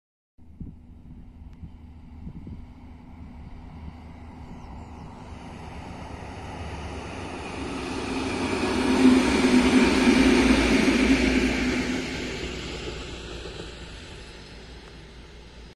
ST48-087+ST48-046 Luzem
ST48-087+ST48-046 jadą luzem w kierunku Stalowej Woli

Lokalizacja:Rejony Przeworsk Gorliczyna - lk68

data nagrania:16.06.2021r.

Pozdrowienia dla maszynisty! 😁